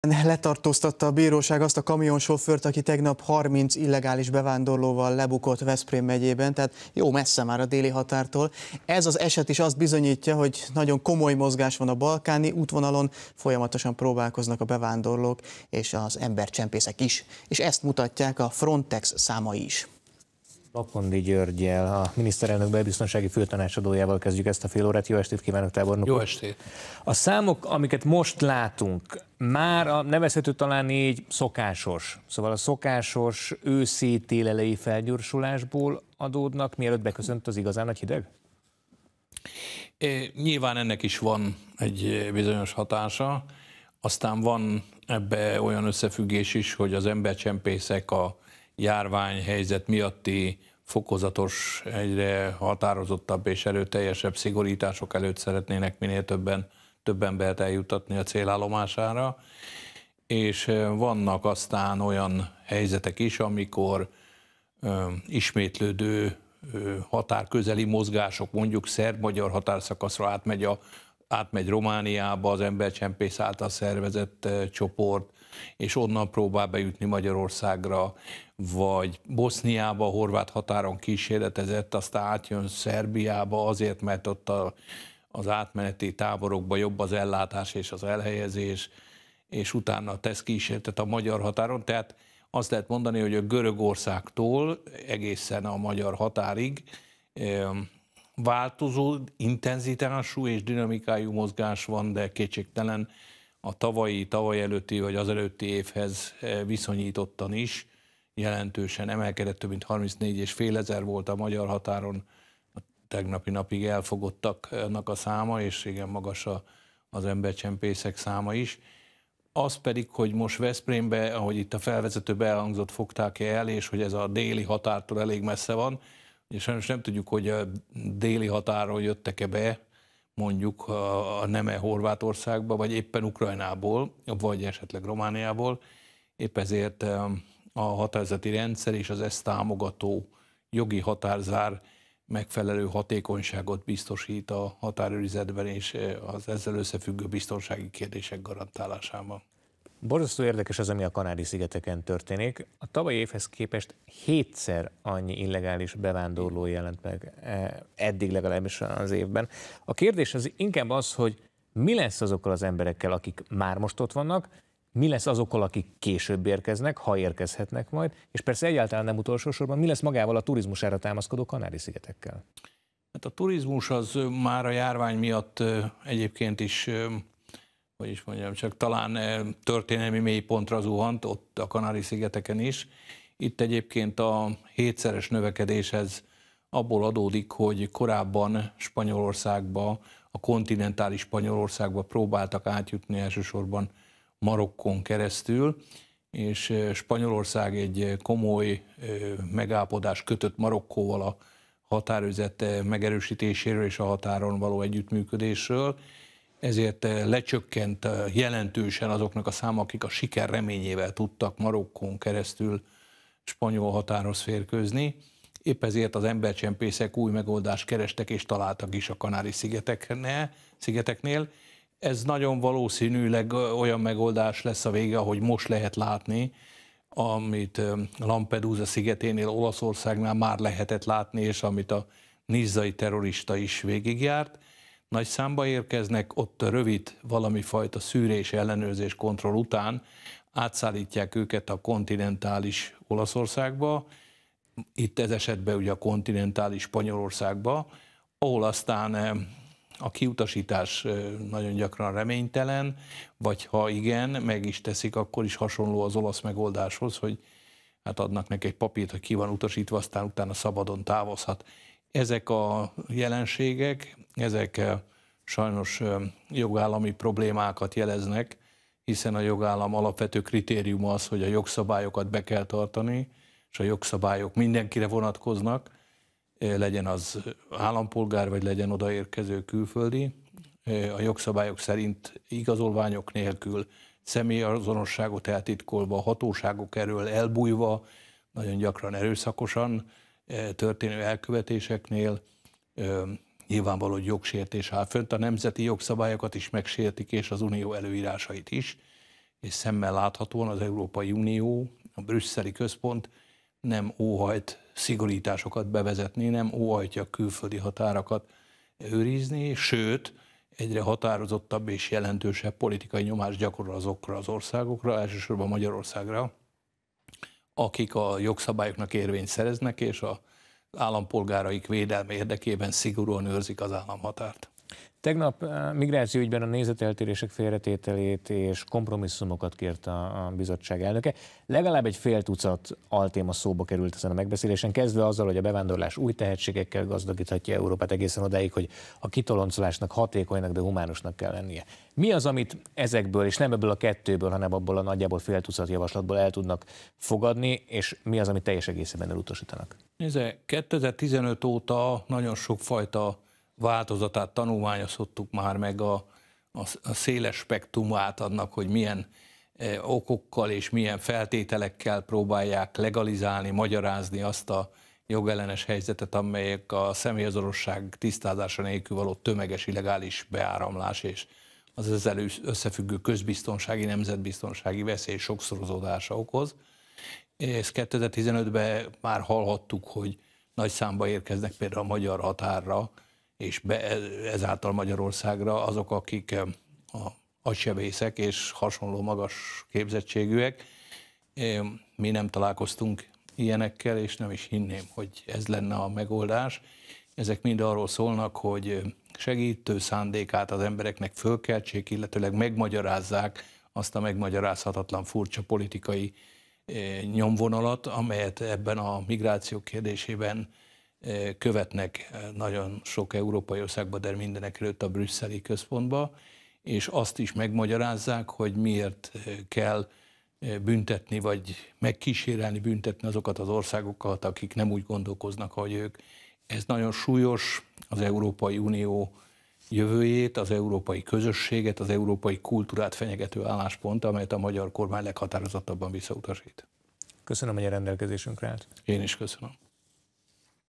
Nőtt az illegális határsértők száma
Letartóztatta ma a bíróság azt a kamionsofőrt, aki tegnap 30 illegális bevándorlóval bukott le Veszprém megyében, tehát jó messze a déli határtól. Ez az eset is bizonyítja, hogy nagyon komoly mozgás van a balkáni útvonalon folyamatosan próbálkoznak a bevándorlók és a csempészek. Ezt mutatják a Frontex számai is. Bakondi György a miniszterelnök belbiztonsági főtanácsadója a vendégünk.